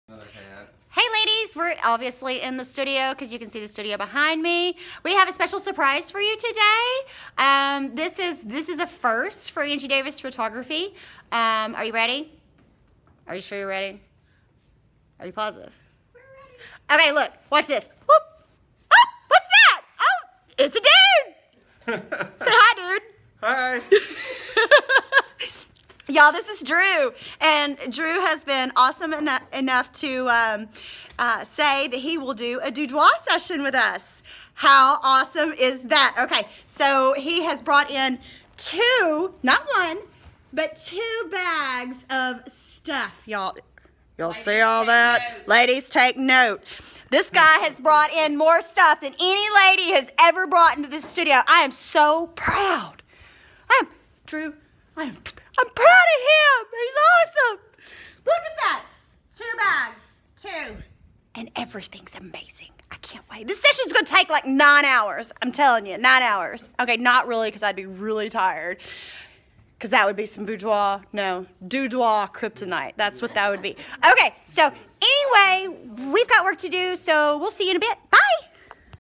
There's A Guy In The Studio Arkansas Boudoir Session
Meet Drew!! We have a Dudoir session today!